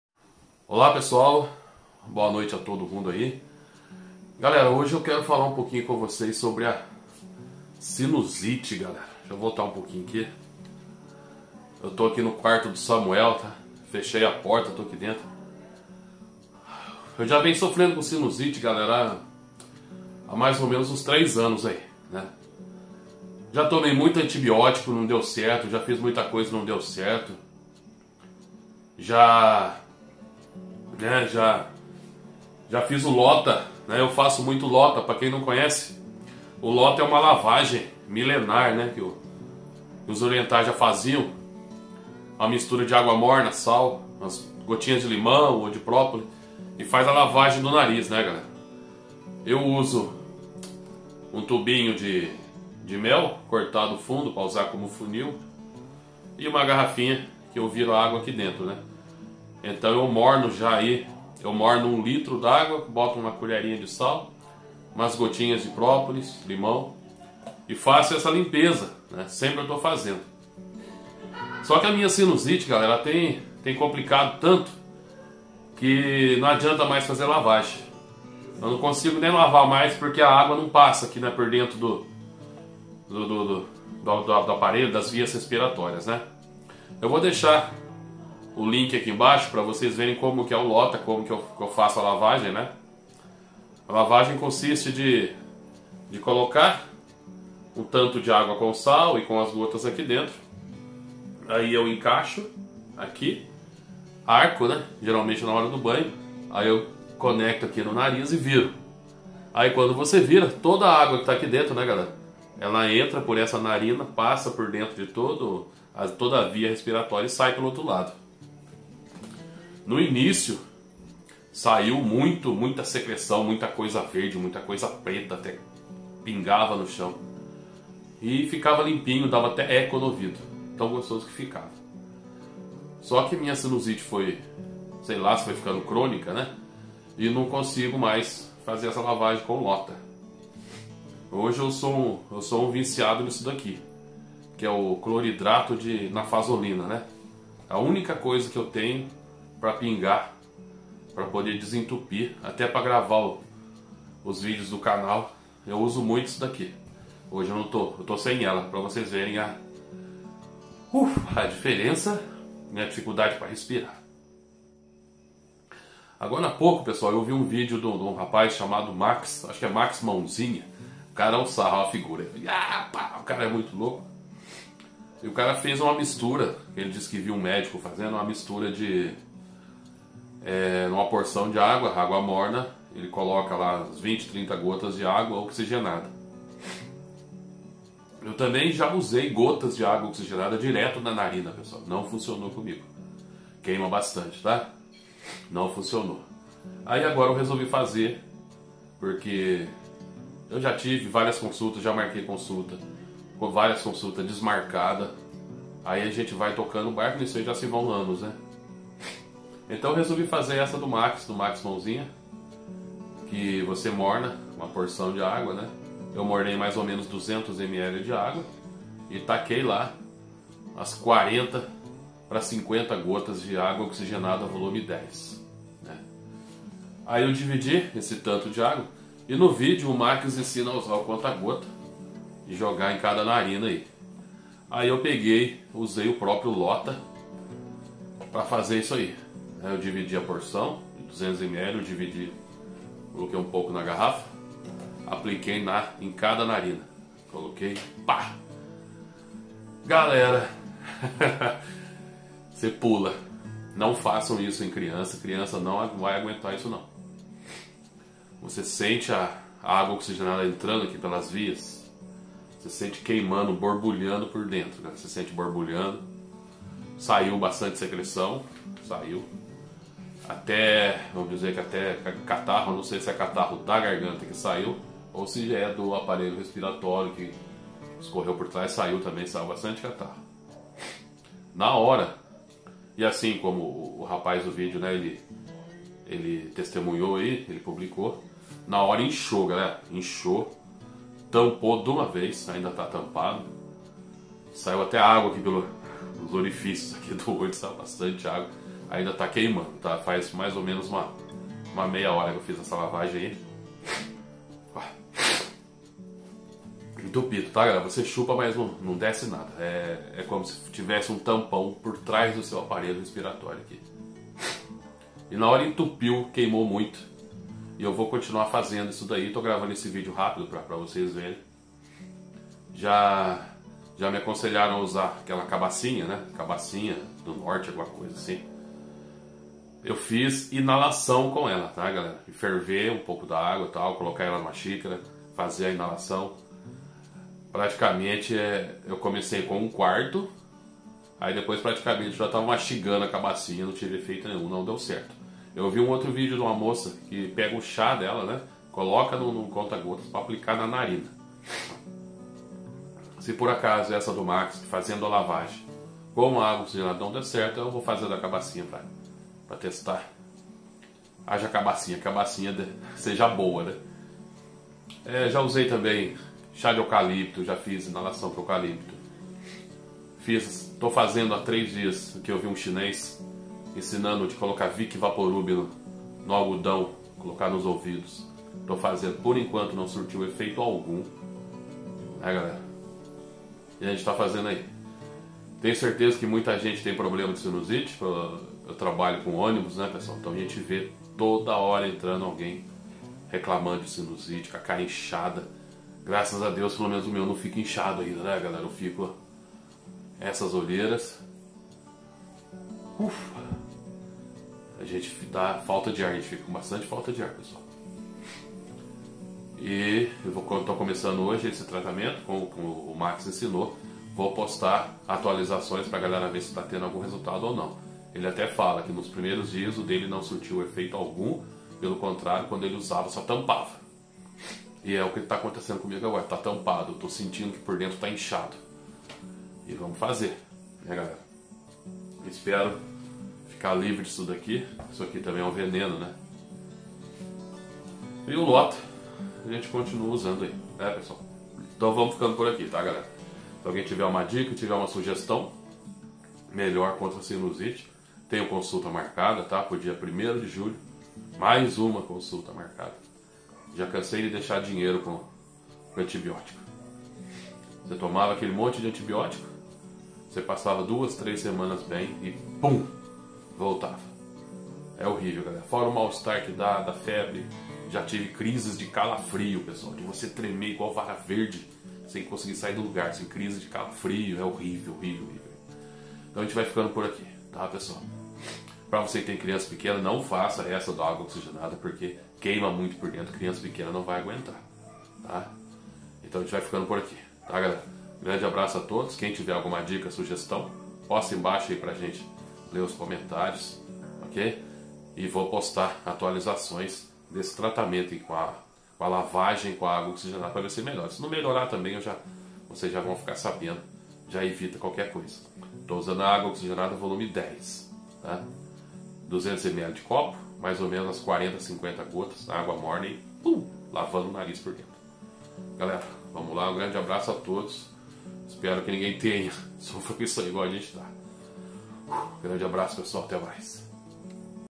● SINUSITE, SOCORRO - NÃO AGUENTO MAIS!!!
- Olá pessoal, nesse momento estou postando um vídeo meio diferente, pois é um problema que venho tendo ha anos e me atrapalha muito!

- Uma SINUSITE do cão, que tem hora que dá vontade de enfiar um tubo no nariz para ver se desentope...

...já tentei de quase tudo; LOTA, inalação com cabacinha, com eucalipto, com Vick...fora a quantidade de antibióticos onde só gastei muito dinheiro e a SINUSITE voltou em poucas semanas...

...procurando muito tratamento alternativo, cheguei no Canal Brasusa, onde estou começando hoje essa aplicação de ÁGUA OXIGENA VOLUME 10 COM ÁGUA MORNA nas narinas, a única diferença é que estou usando o procedimento do LOTA para aplicar essa água direto nas vias!!!

- SEGUE ABAIXO OS LINKS DOS VÍDEOS ONDE JÁ FAÇO LAVAGEM COM O LOTA E O DOS CANAIS BRASUSA E DO DOUTOR QUE ELE MENCIONA:

...vou ficando por aqui pessoal, agradecendo a todos, aceitando dicas e convidando a acompanharem o resultado!!!

- Abração e que Deus os abençoe sempre!!!